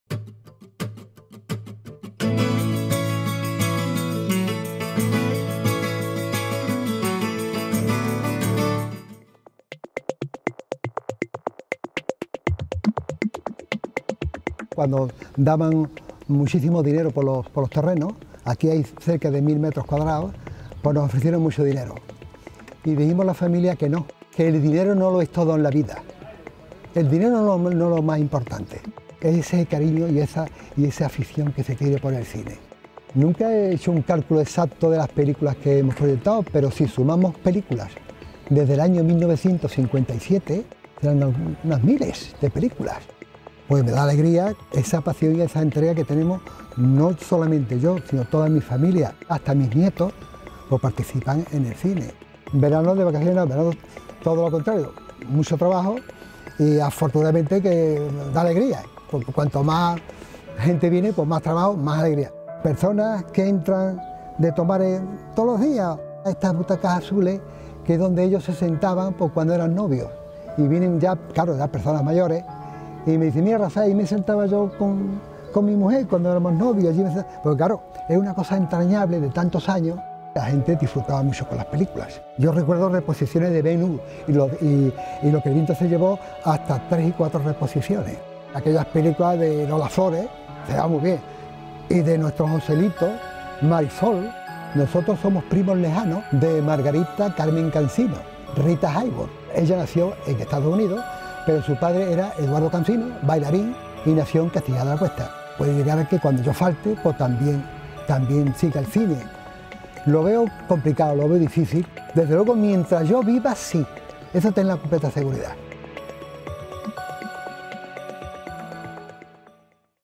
HDL Rafael Cansino, cine bajo las estrellas con más de medio siglo de historia
Una gran pantalla de 72 metros cuadrados. Un recinto de casi mil con ambigú. Sillas, un proyector y la cúpula de estrellas sobre los espectadores. Es Cinema Tomares, el último cine privado de la provincia de Sevilla, fundado en 1964. Su artífice es Rafael Cansino, un hombre de 70 años que heredó su inmensa pasión por el cine de su padre. Rafael creció en una sala de proyección, como el protagonista de ‘Cinema Paradiso’. Y ahí sigue. Confiesa una íntima emoción al volver a ver las películas de Margarita Carmen Cansino, con quien tiene una lejana relación de parentesco. Se trata de la estrella universal conocida como Rita Hayworth.